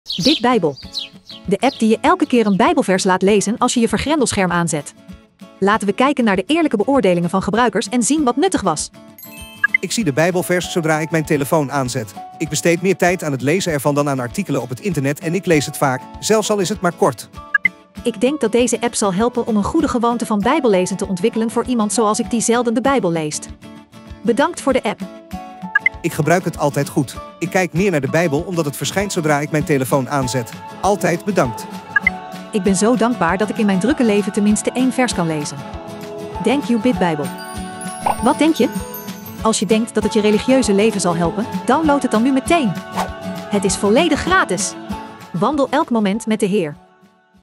(BB)NL2↔️BitBijbel Recensie Aflevering 2
BitBijbel

Een app die je laat lezen één Bijbelvers elke keer als je je vergrendelscherm aanzet, Echte recensies van BitBijbel!

Ik zie de Bijbelvers zodra ik mijn telefoon aanzet.
Ik besteed meer tijd aan het lezen ervan dan aan artikelen op het internet en ik lees het vaak, zelfs al is het maar kort.

Ik denk dat deze app zal helpen om een goede gewoonte van Bijbellezen te ontwikkelen voor iemand zoals ik die zelden de Bijbel leest. Bedankt voor de app.

Ik gebruik het altijd goed. Ik kijk meer naar de Bijbel omdat het verschijnt zodra ik mijn telefoon aanzet. Altijd bedankt.

Ik ben zo dankbaar dat ik in mijn drukke leven tenminste één vers kan lezen. Dank U BitBijbel!

Wat denk je? Als je denkt dat het je religieuze leven zal helpen, download het dan nu meteen! Het is volledig gratis!